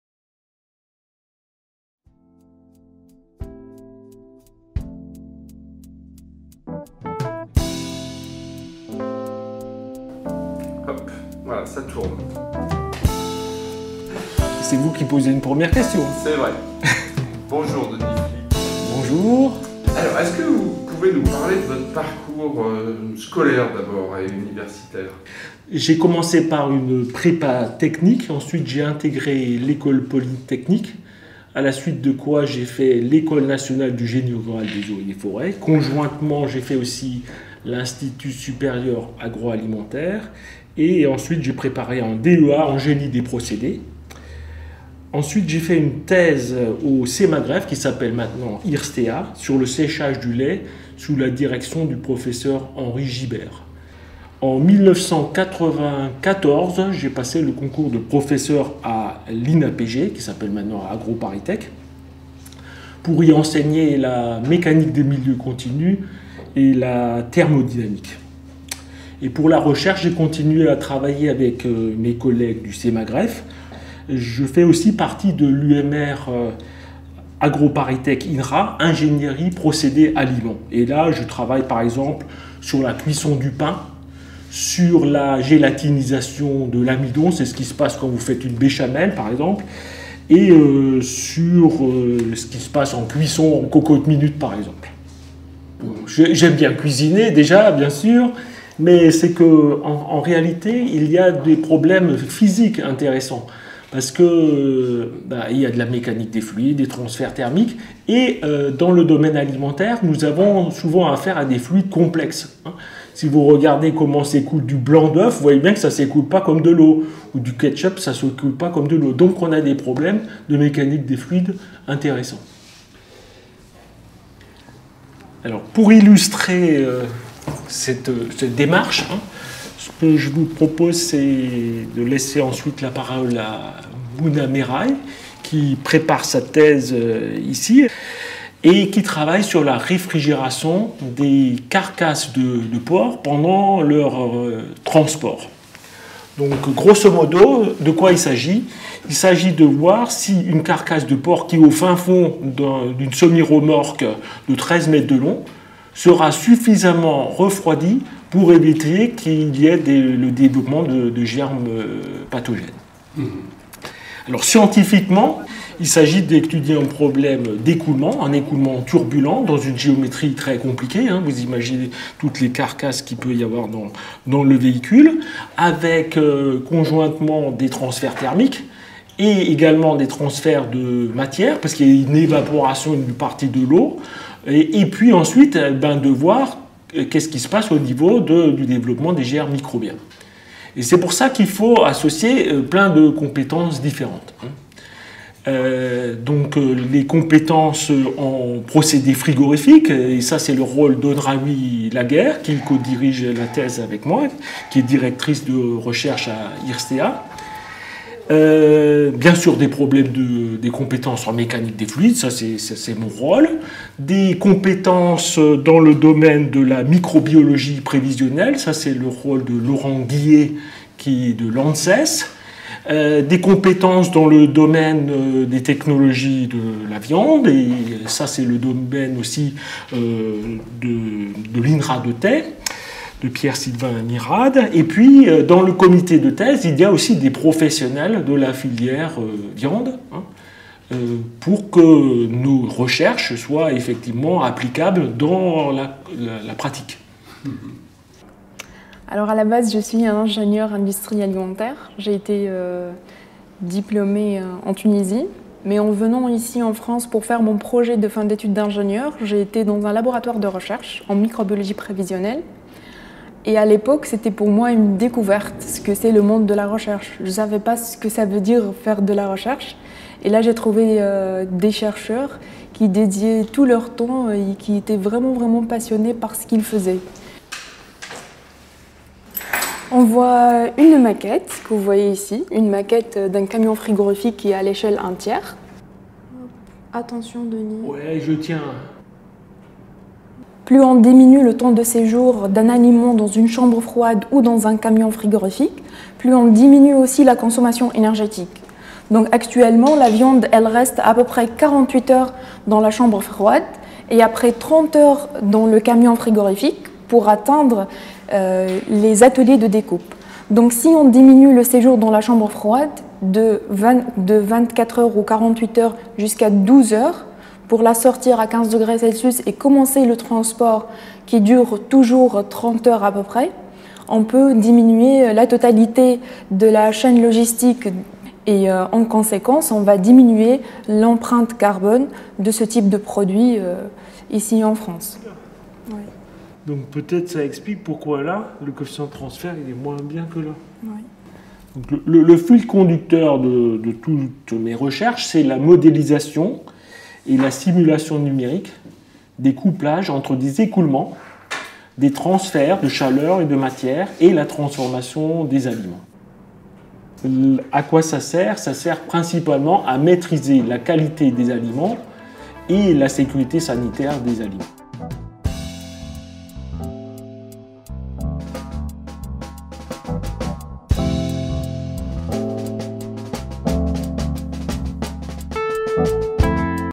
Portraits de Recherches - Denis Flick - Mécanique des fluides
Avec les Portraits de Recherches, AgroParisTech donne à voir la diversité d’une recherche finalisée et tournée vers la contribution aux enjeux de société.

Denis Flick, professeur à AgroParisTech spécialiste de la mécanique des fluides (UMR GENIAL - Ingénierie Procédés Aliments), présente, avec  Mouna Meraï, l’étudiante en thèse, une étude sur l’écoulement de l’air dans les camions frigorifiques. Cette recherche vise à améliorer la sécurité sanitaire des aliments transportés.
Les productions en accès ouvert de Denis Flick via HAL-AgroParisTech : cliquez ici et cliquez ici